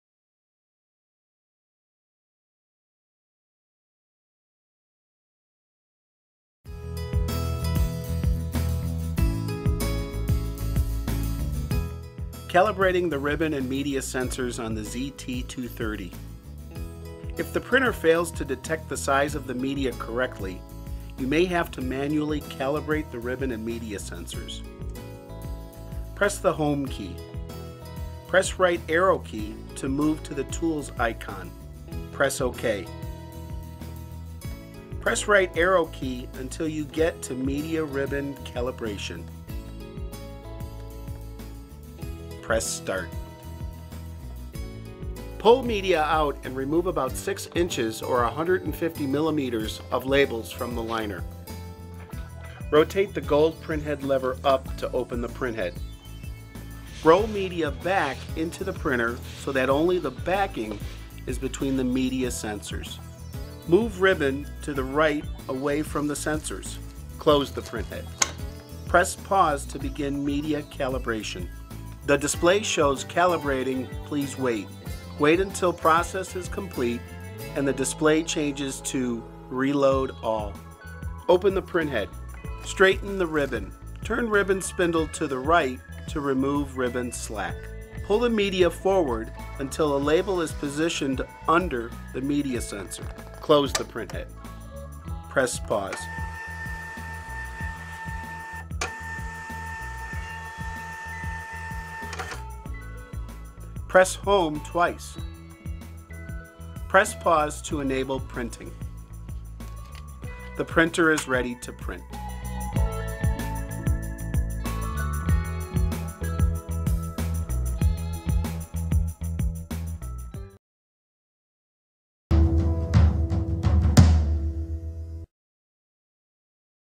Zebra ZT230 Manually Calibrate Ribbon and Media Sensors
Learn How to Calibrate (Manually) Ribbon and Media Sensor on Zebra ZT230 Industrial Barcode Printer